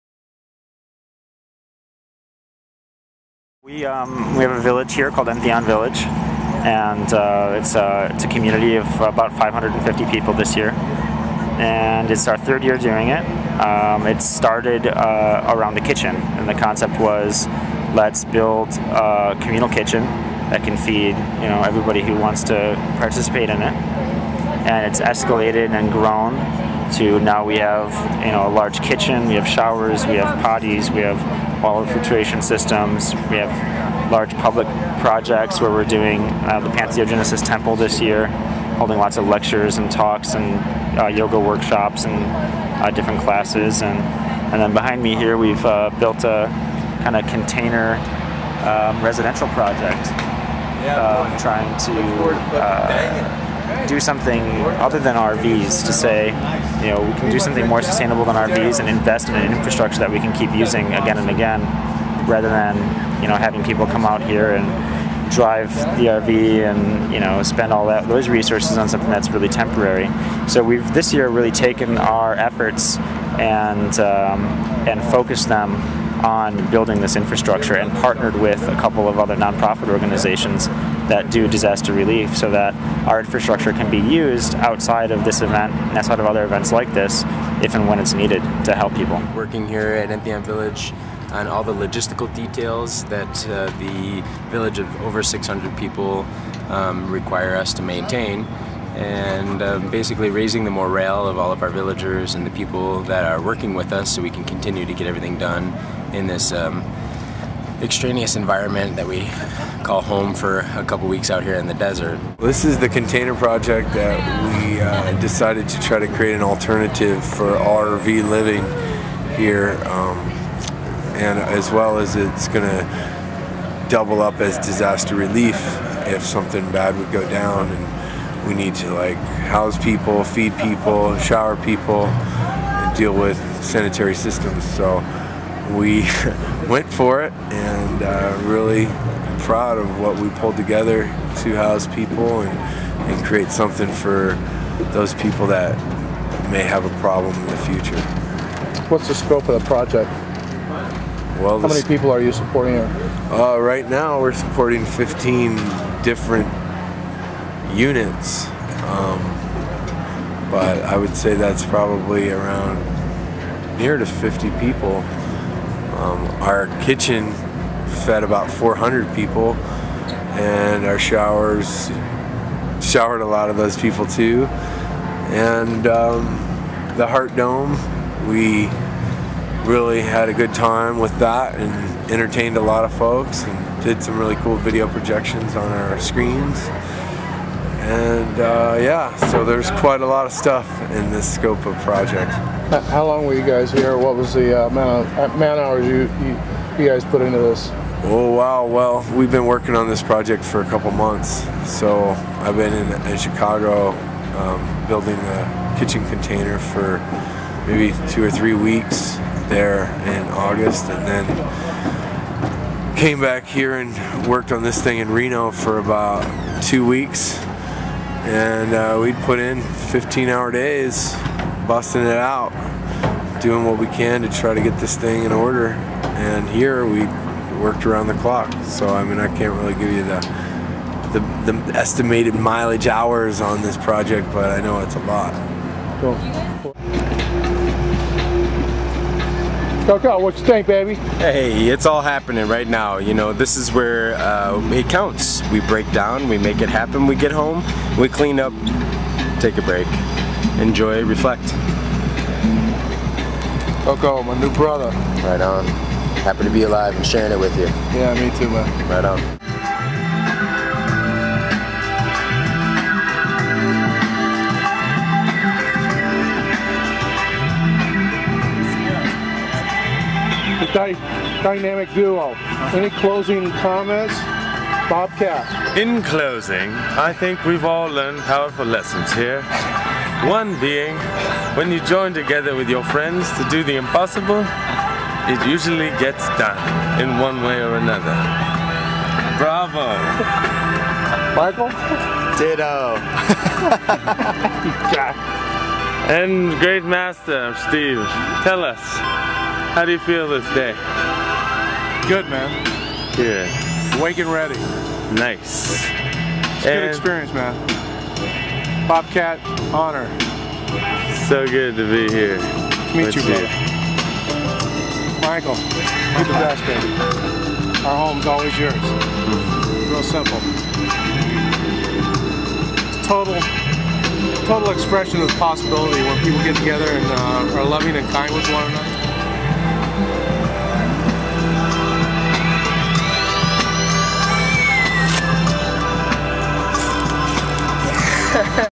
Entheon Village at Burning Man 2008
Matt Atwood, Coco, Bobcat, and Michael Kang describe the Entheon Village shipping container project at Burning Man 2008.